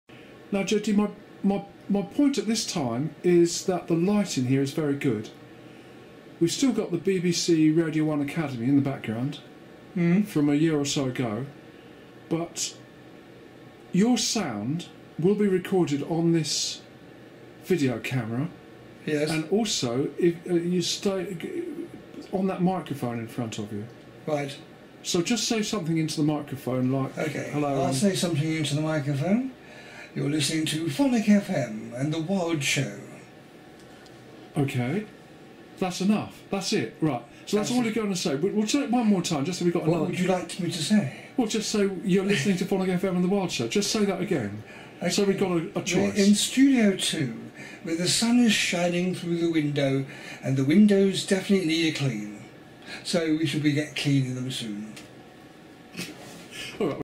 JD on camera in Studio Two
Test of lighting in studio Two is ok, sound reasonable see two other clips with sound as recorded on studio computer.